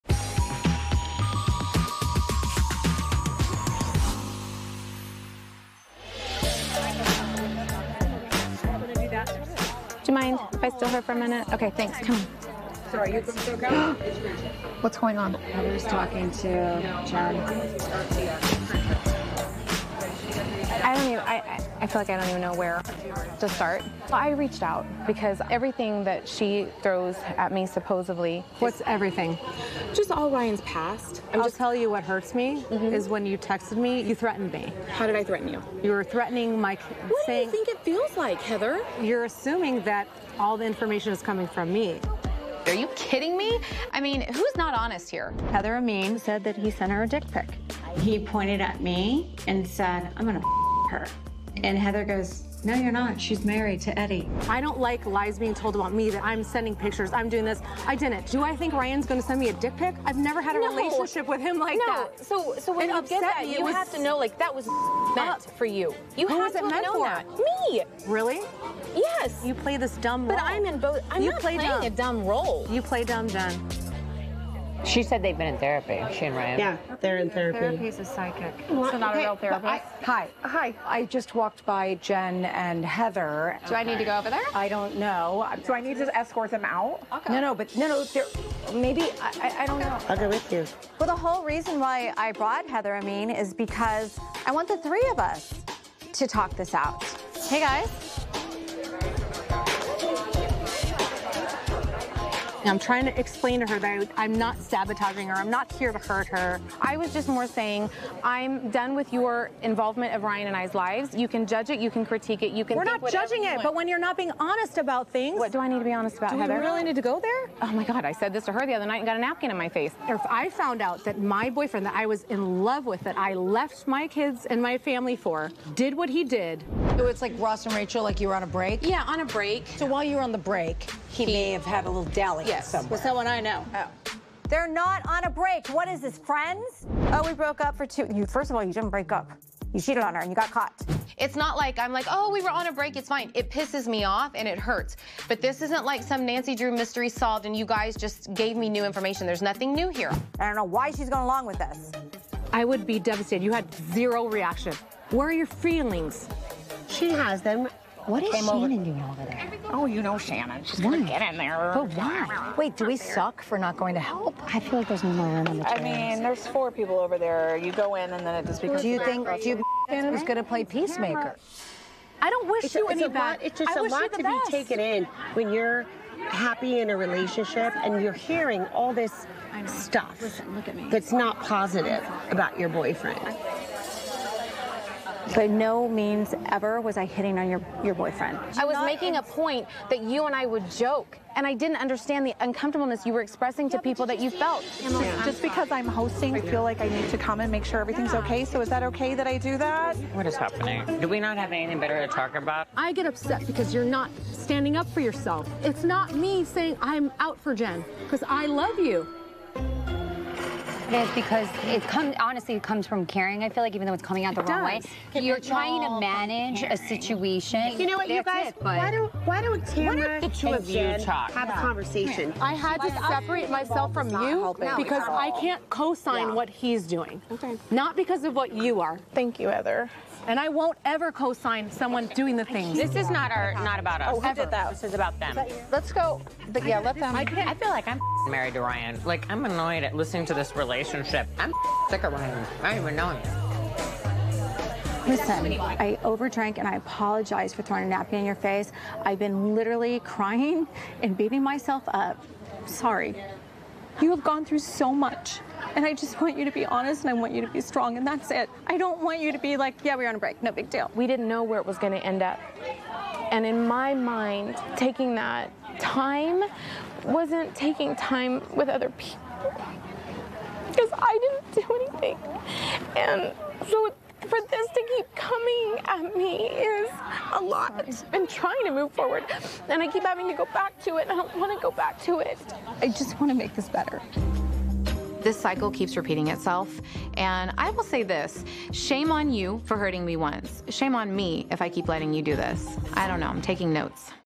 Jenn confronts her friend Heather about her talking bad about her boyfriend (S17, E8)
Airs every Wednesday at 8/7c on Bravo and streaming the next day on Peacock.